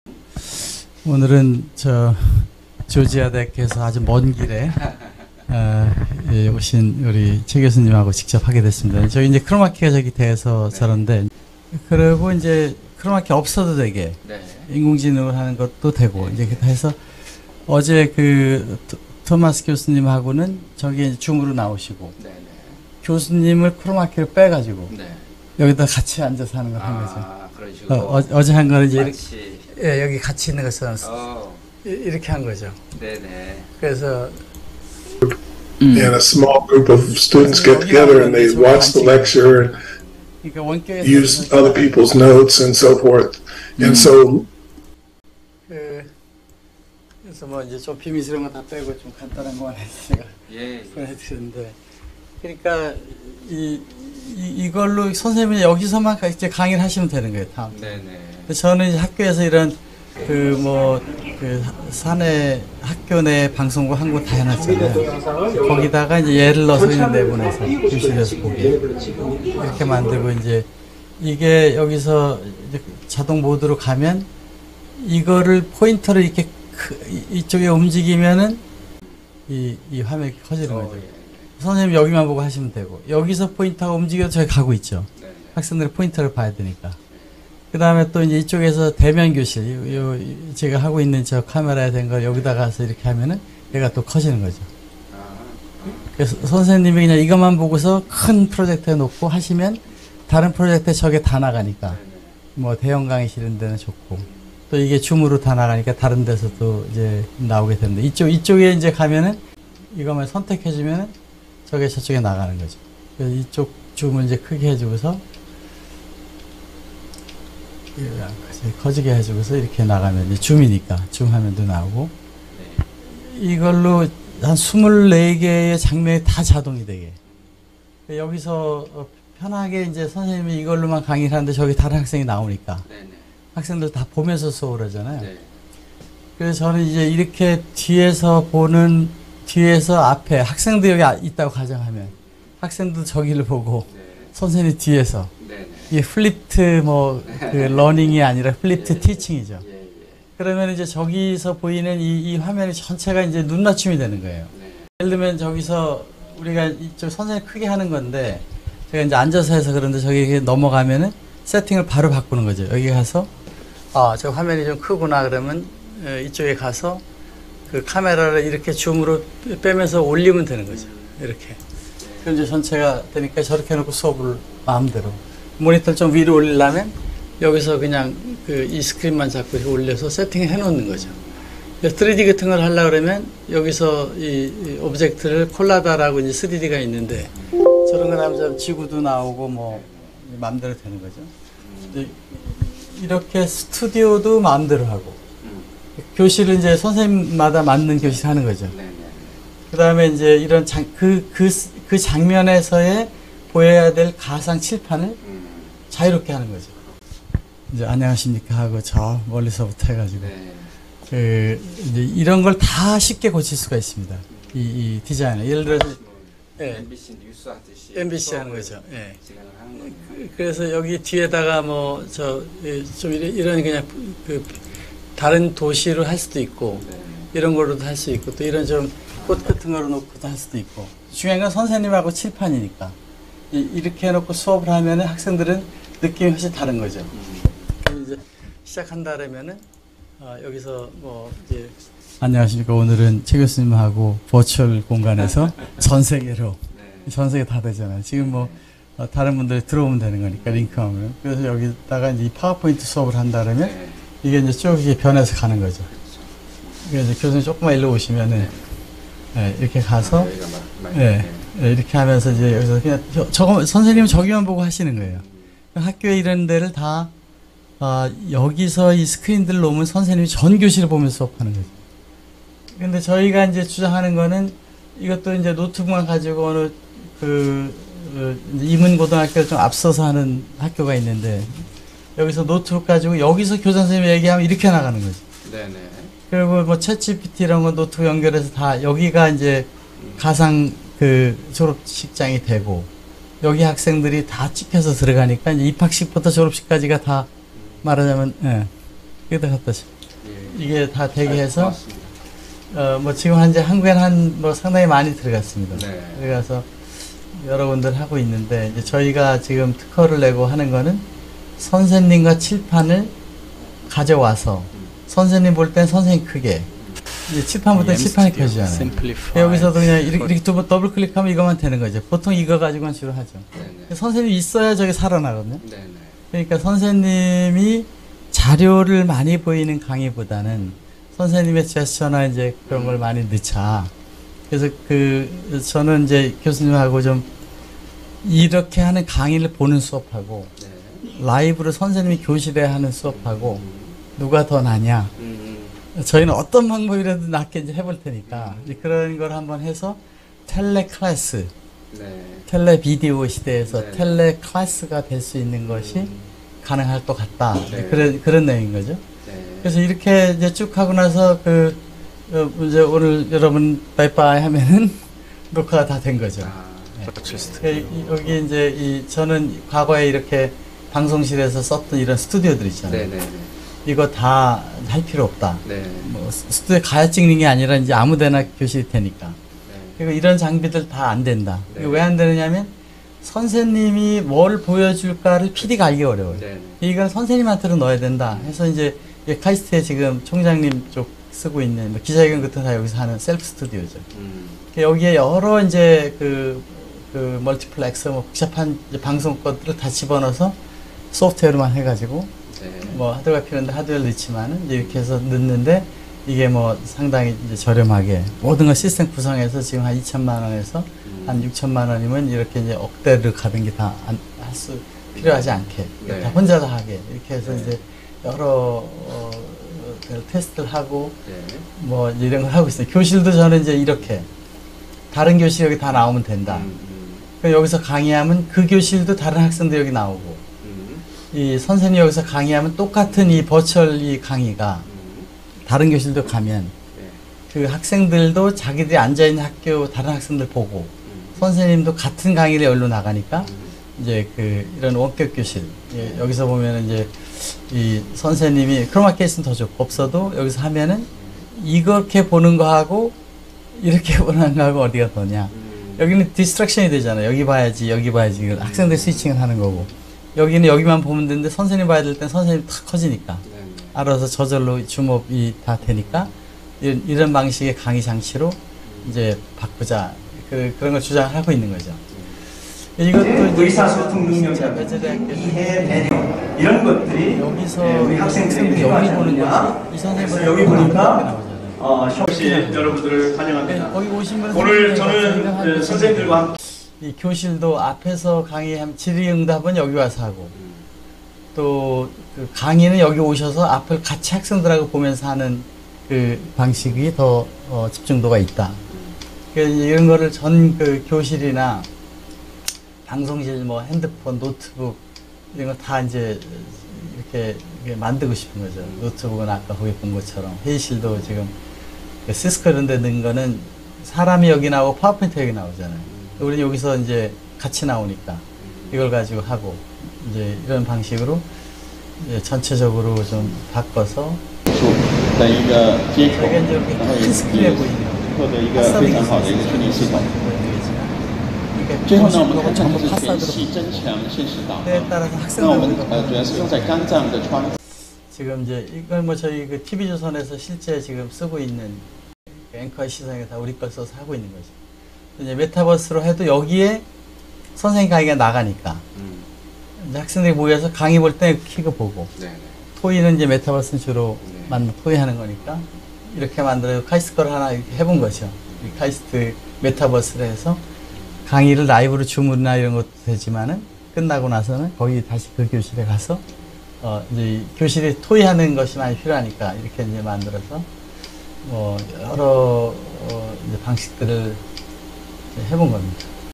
조자아대학 교육학 최교수님 다림 방문 데모 - 스마트 교실과 교육 혁신 협의
iStudio의 교실, 노트북, 스튜디오, 방송실에 1인 강의 장치로 사용하기 위한 새로운 방법들에 대한 가상교실 제작, 가상카메라 세팅 부터 인공지능 배경 스튜디오 기능 처리를 통한 언제 어디서나 방송국이 되는 새로운 iStudio 스마트 강의 장치와 교실, 교육에 대한 협의를 교육학 전문가님과 나눈 이야기